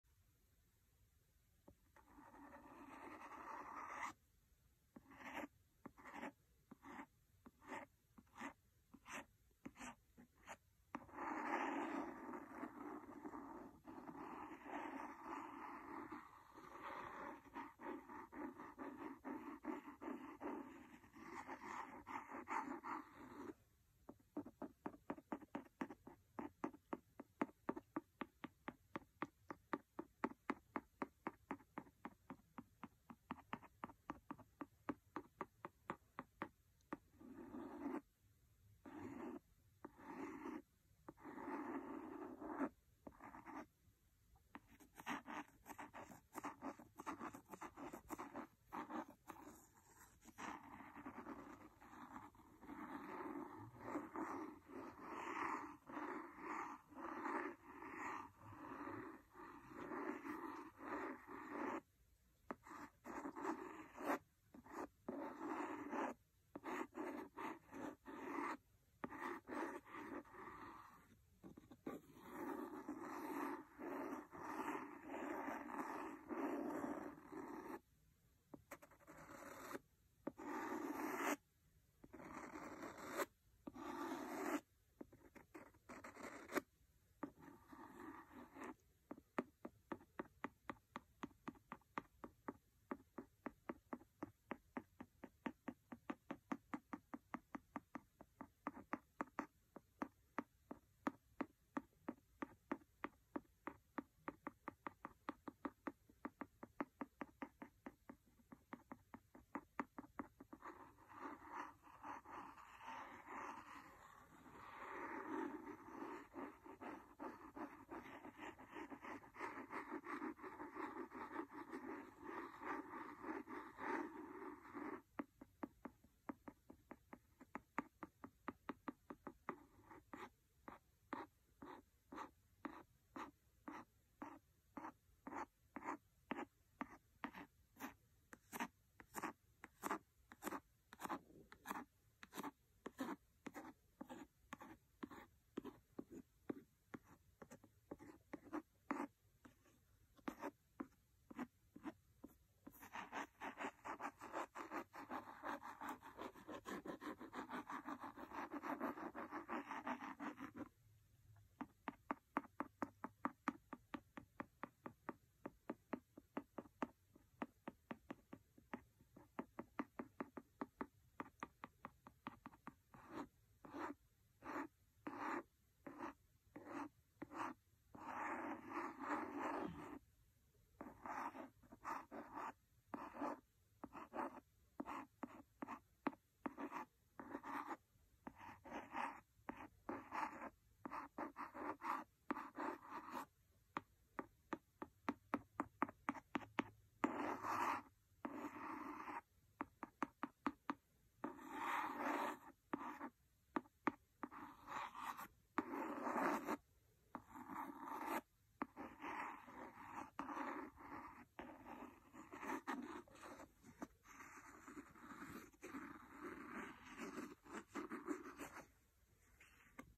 Book Scratching and Tapping ASMR
In this video I use a pencil to scratch and tap a book! Enjoy 💚